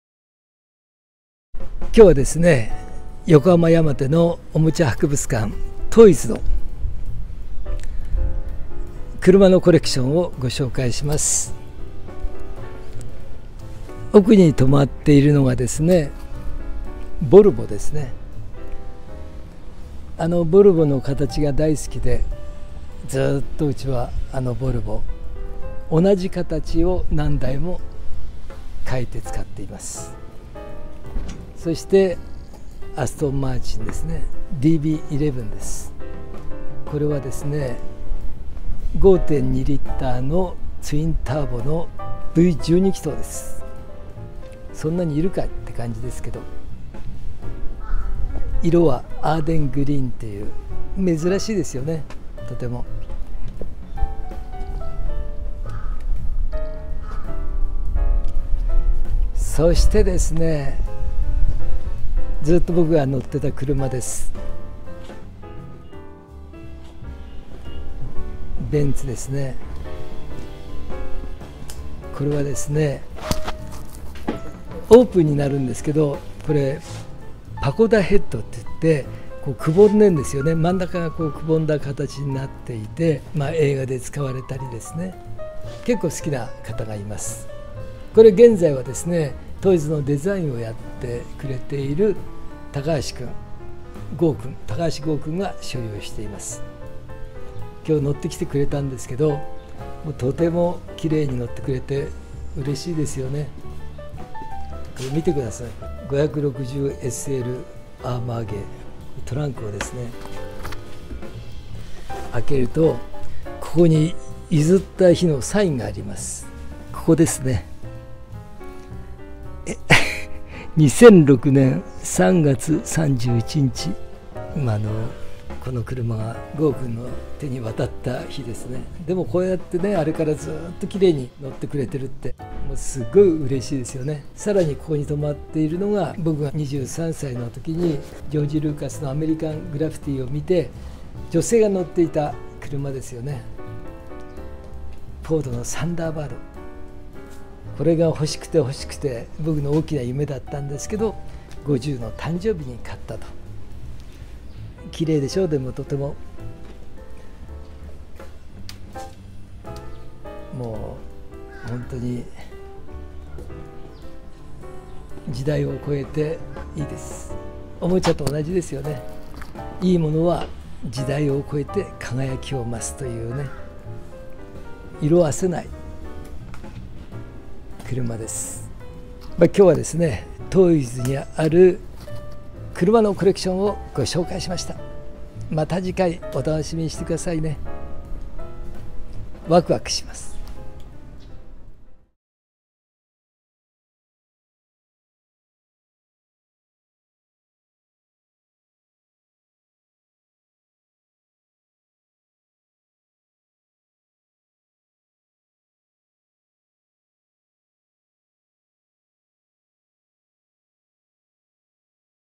北原照久 愛車遍歴
【KITAHARA COLLECTION CHANNEL Vol.579】
今回のYouTubeは横浜・山手より車のコレクションをご紹介します。
トイズのデザインなどをお願いしている高橋剛さんの愛車は
ベンツの560SLAMG。この車、以前は北原の愛車でした。
パゴダヘッドという特徴的な形のルーフ。
トランクを開けるとお譲りした日付とサインがあります。
今でも綺麗に乗ってくれているのが嬉しいと北原は喜んでいます。
「いい物は時代を超えて輝きを増す」という言葉がぴったりですね。

・YouTubeチャンネルVol.555